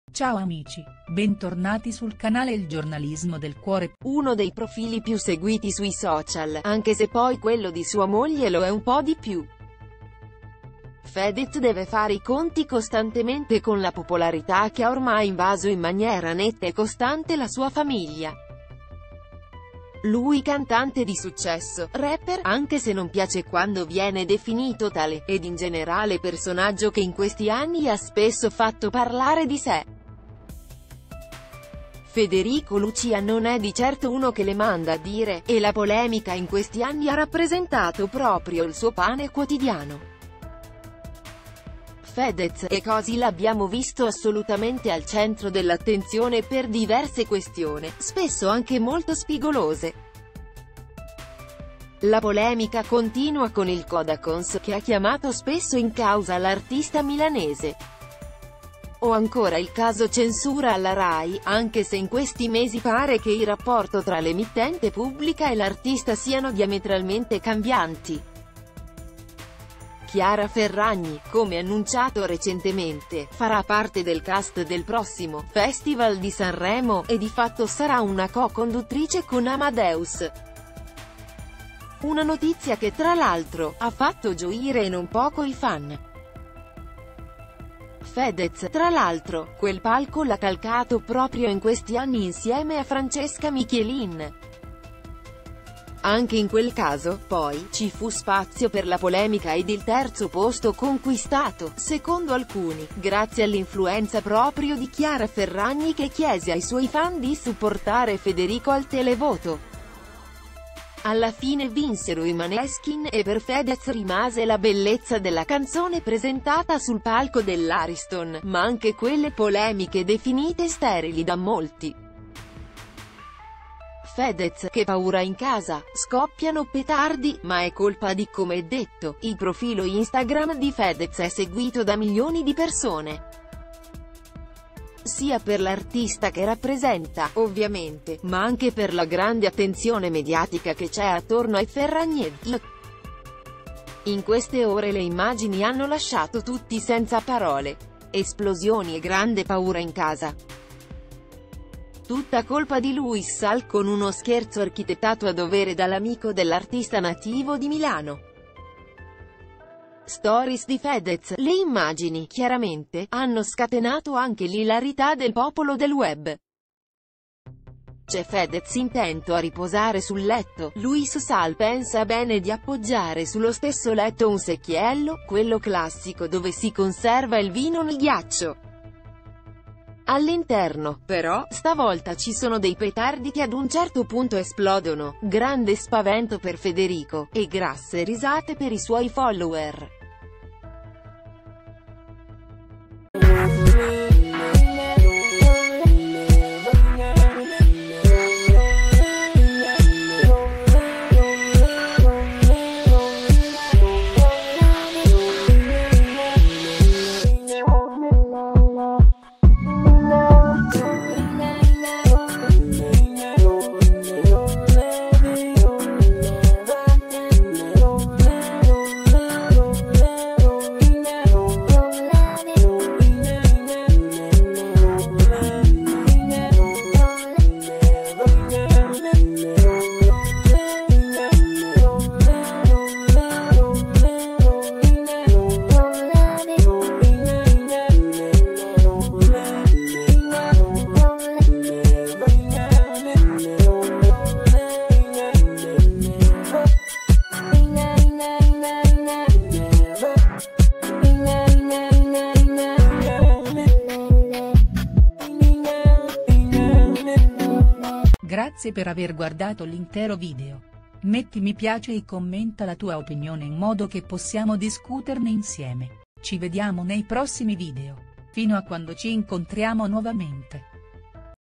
Fedez, spavento e paura in casa è accaduto proprio in queste ore
Fedez, che paura in casa. Esplodono dei petardi, lui si spaventa. Le immagini sono state pubblicate sul web.